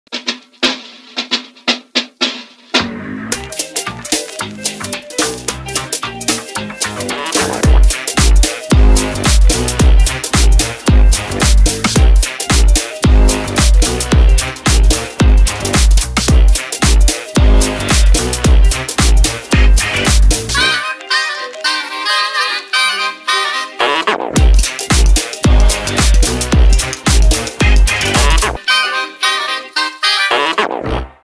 G FUEL Flavor Mix | Strawberry Shortcake
Did you know you can mix G FUEL Energy flavors to unlock hidden flavors?

Try this: 1 scoop Strawberry Shortcake + 12-16 oz cold milk = Strawberry Shortcake! 🍓🍰🥛